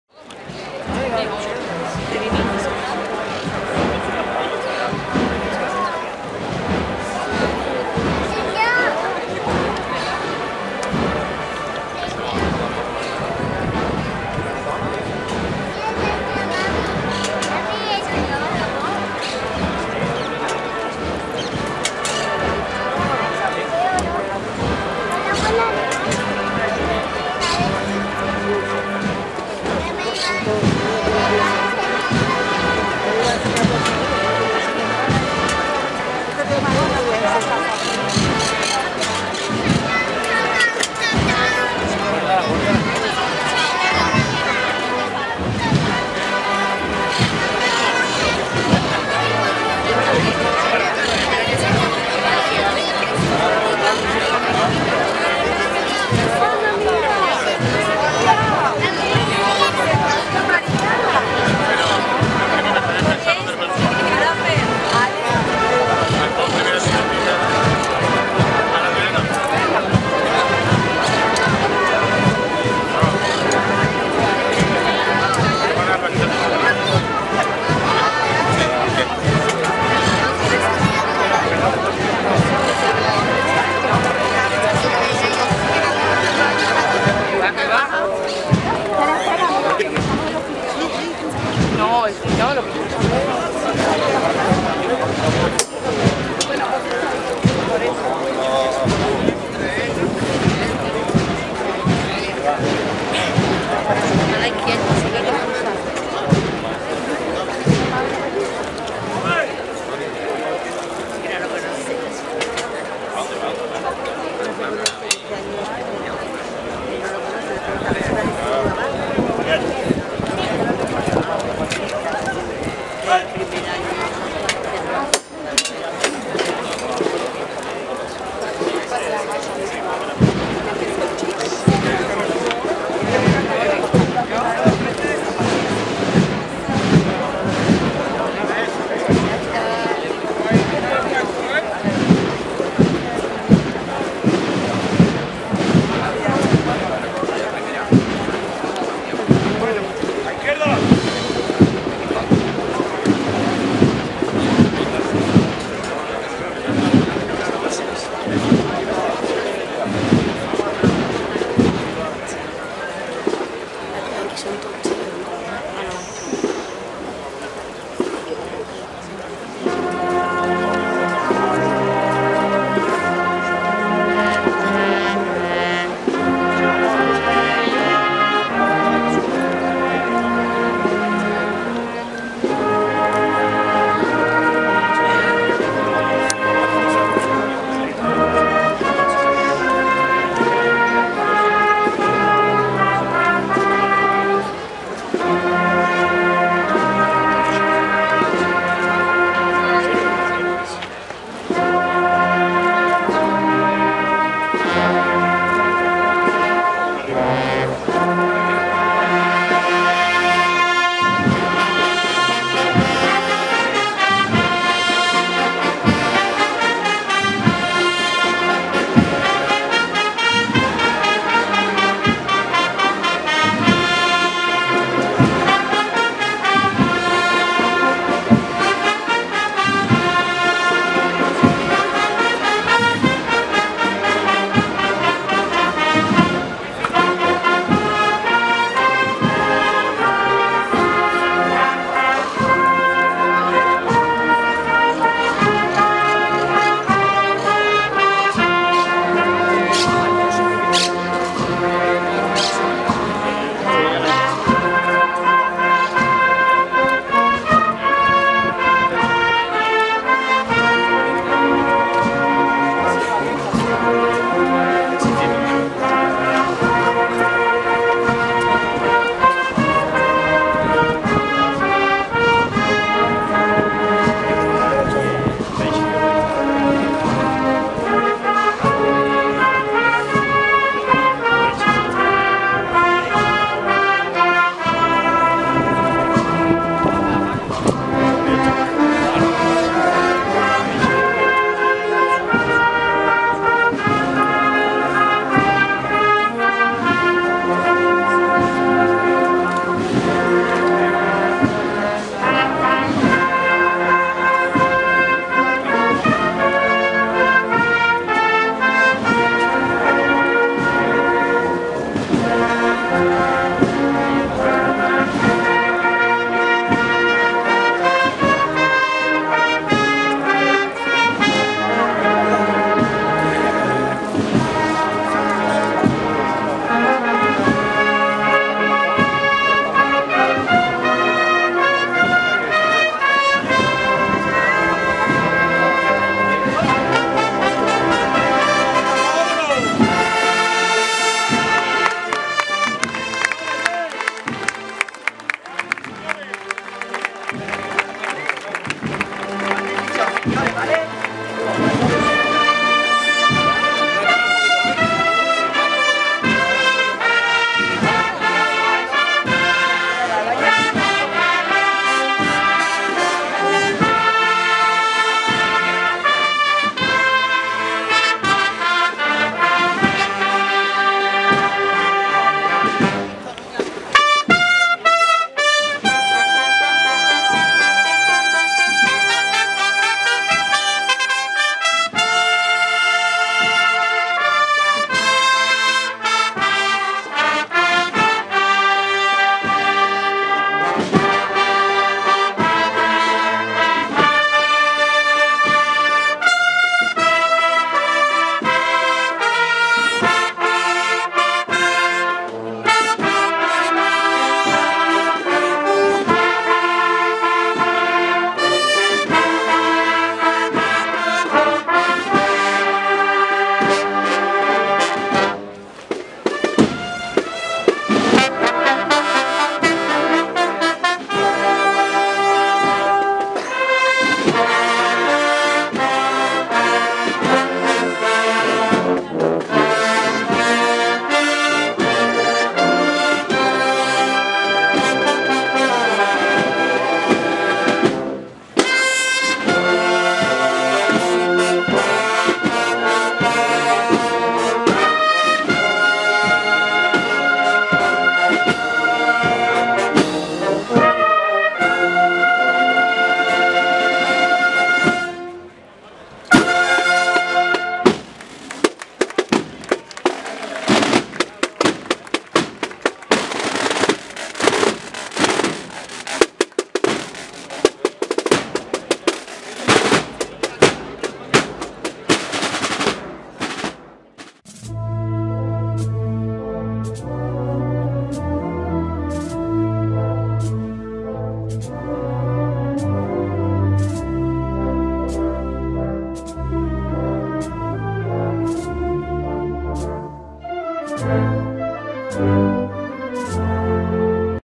CRISTO DE LA HUMILDAD (Sevilla Este) - C/La acogida
Salida de Nuestro Padre Jesús de la Humildad, de la Agrupación Parroquial La Espiga, del barrio de Sevilla Este, en su primera e histórica salida procesional. Siendo además la primera procesión del barrio. Acompaña musicalmente tras el misterio la Agrupación Musical Nuestra Señora de la Encarnación de San Benito.